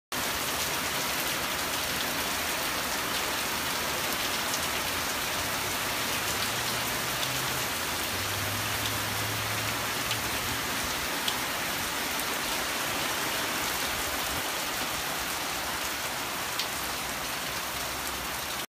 Kauai rain
Leaving a very dry Oregon, to be greeted by the refreshing Kauai rain